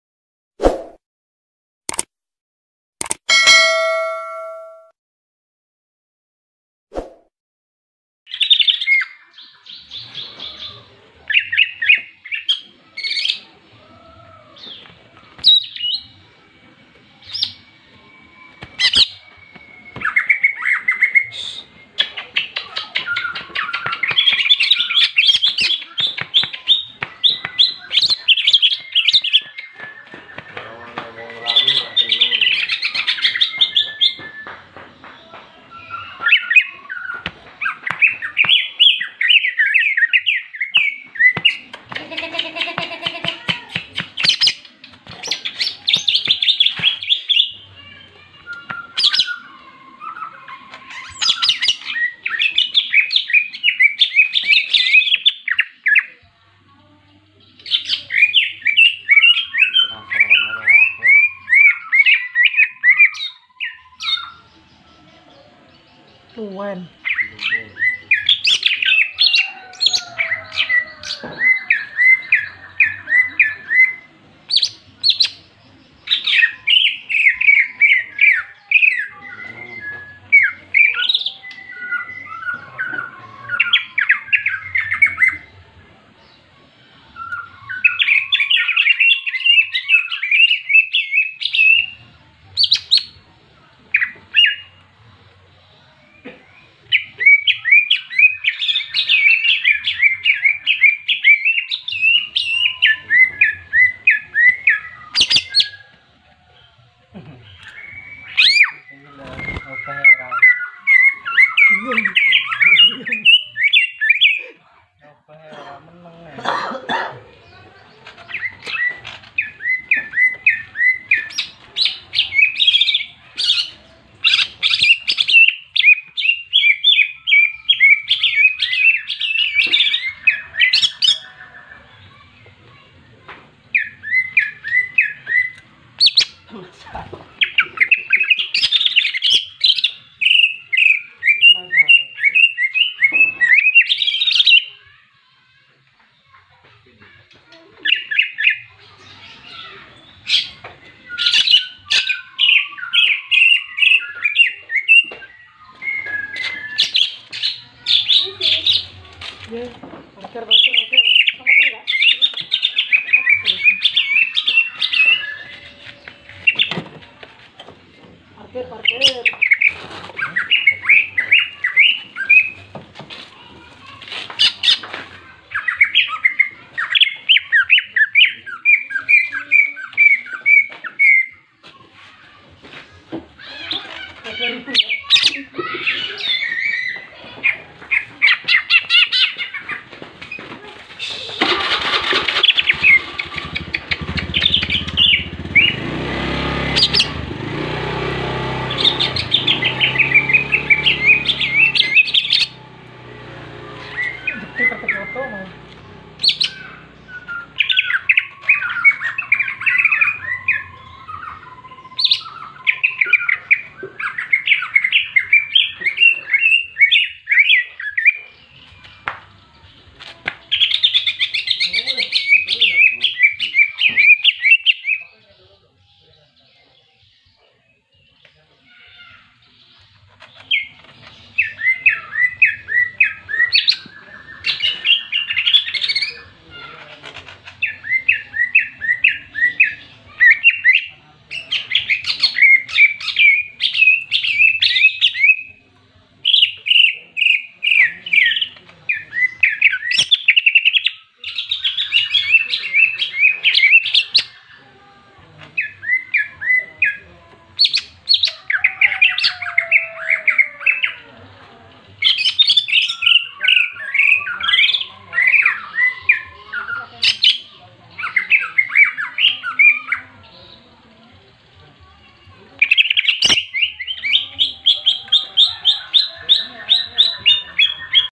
CUCAK IJO TARUNG FULL NGENTROK JENGGRIK DI JAMIN BIKIN EMOSI CUCAK IJO LAIN
Video ini kami harapkan untuk merangsang bunyi burung CUCAK IJO anda. Syukur - syukur burung CUCAK IJO anda bisa bertambah gacor